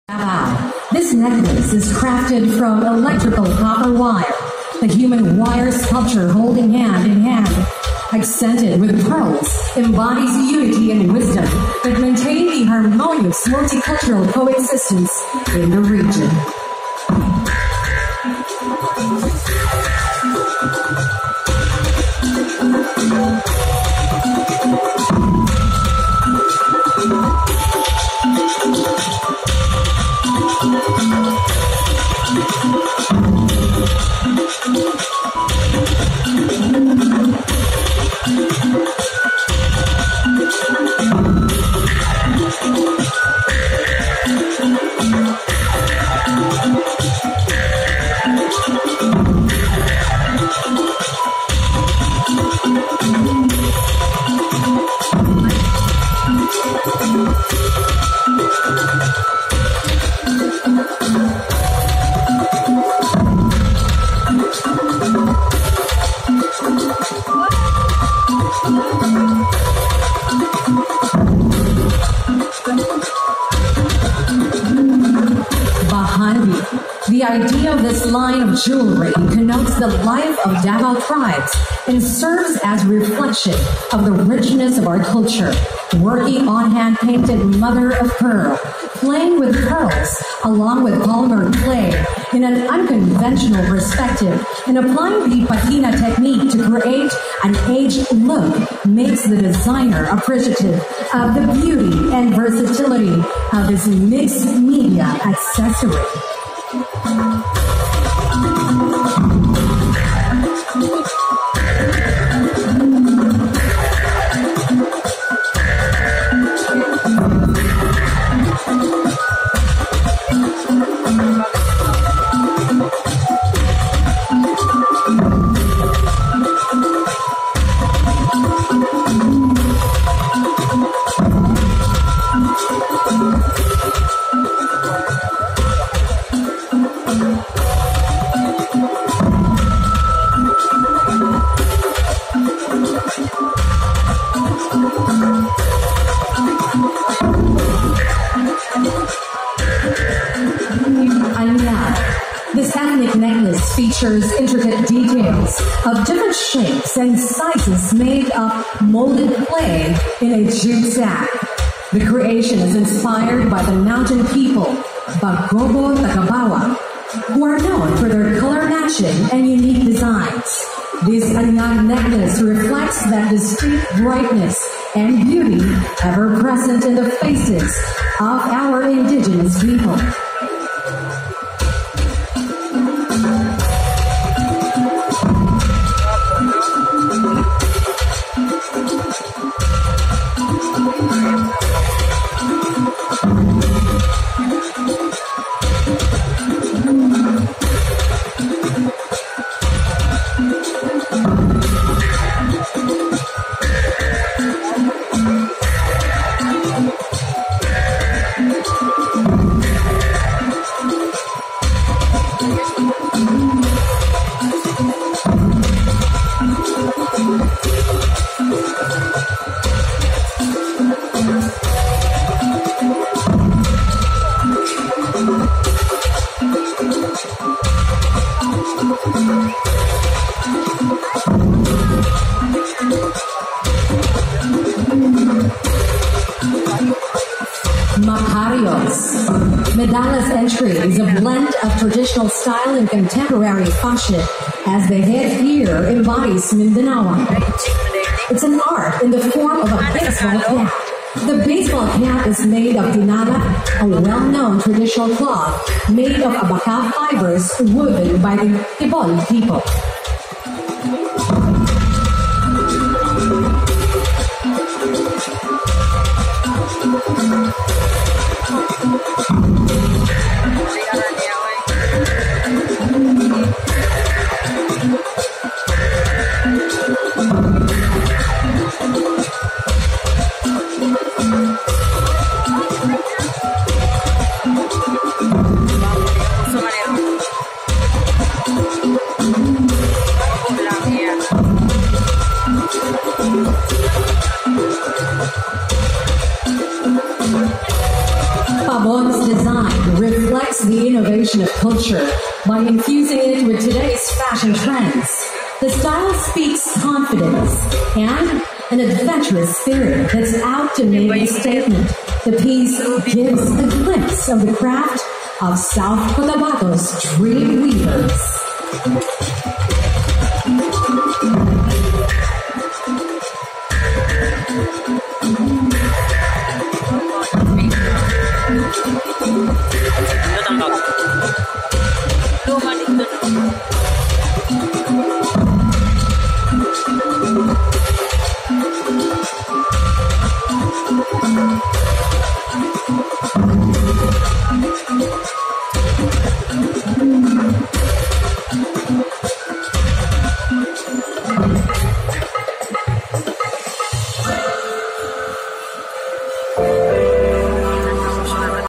Davao Fashion Week (Habi kadayawan) part 2
A joint project of the Davao LGUs Kadayawan Executive Committee and the Davao Fashion and Design Council Foundation, Inc. (DFDCFI), Habi Kadayawan 2019 paid homage to the journey of Davao’s 11 migrant tribes across the diverse terrain of their ancestral homes in Mindanao to a newfound home in Davao City.

at Ayala abreeza mall bajada davao city